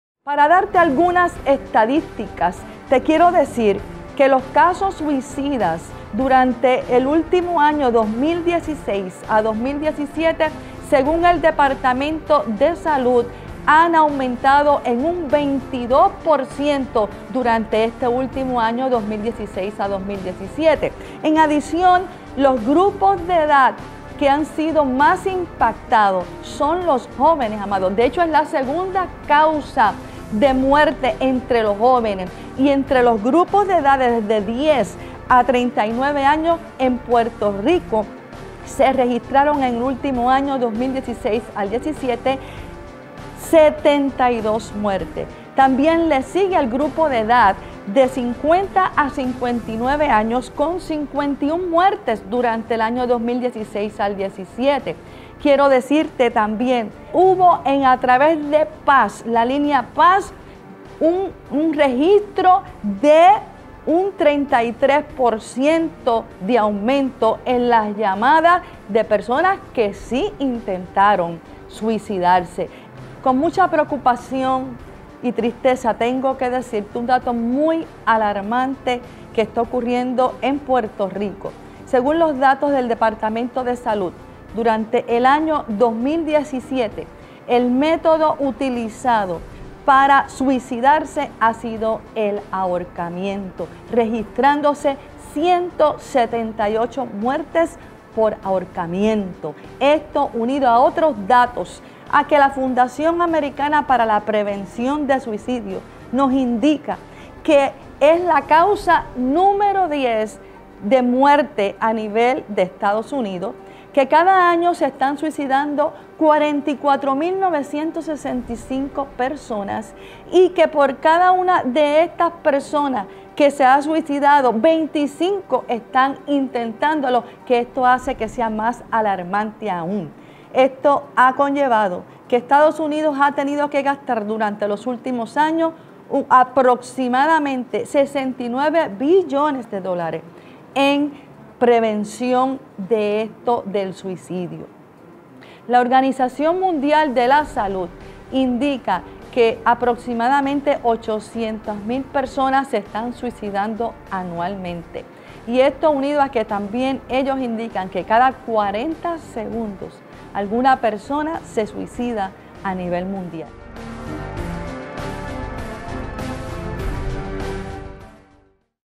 Estadísticas sobre el suicidio en Puerto Rico, Estados Unidos y en el mundo.
Corta presentación sobre estadísticas del suicidio categorizadas en tendencias por grupo de edad, intentos suicidas, métodos utilizados y estadísticas generales sobre el suicidio en Puerto Rico, los Estados Unidos y en el mundo.